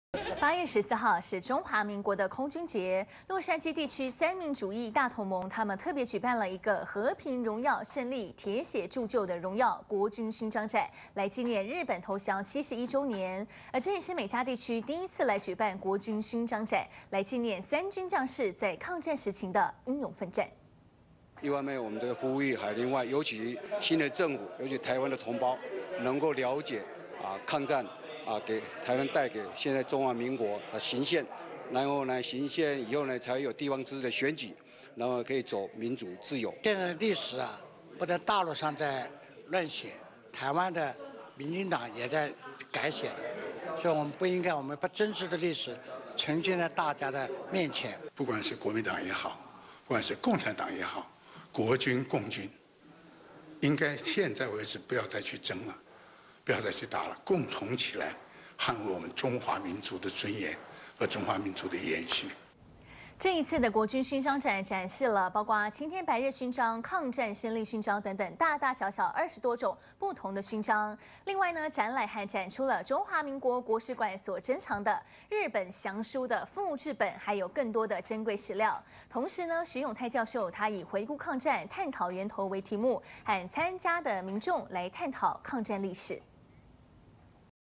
「國軍勳章展」 呼籲海外華僑記住歷史
八月十四日是中華民國的空軍節，洛杉磯地區三民主義大同盟，特別舉辦，「和平·榮耀·勝利--鐵血鑄就的榮耀-國軍勳章展」，來紀念日本投降七十一週年。這是美加地區第一次舉辦國軍勳章展，來紀念三軍將士在抗戰時的英勇奮戰。